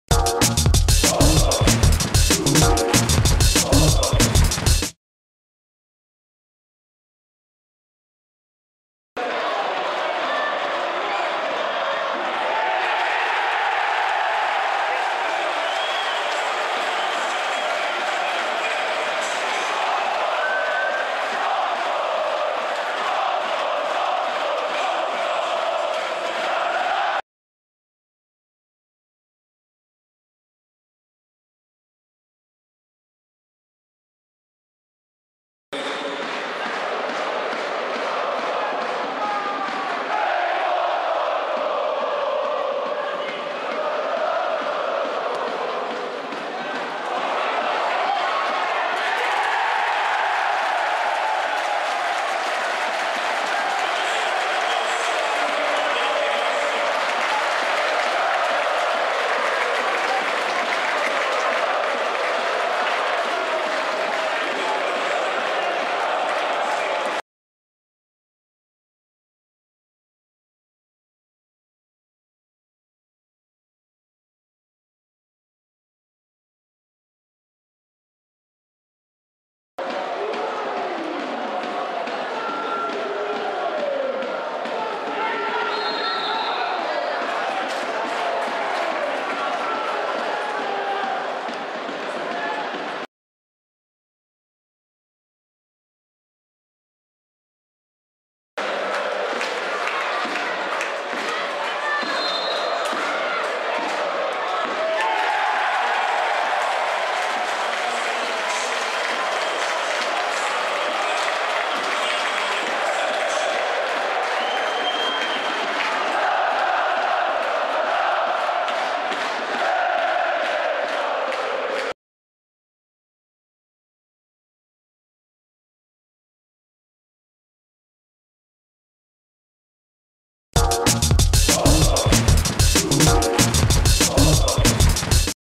Bramki z meczu derbowego: Lublinianka - Motor 0:3 (0:1)
Dwa trafienia Artura Gieragi i piękna bramka Damiana Falisiewicza dały Motorowi cenną wygraną w Derbach Miasta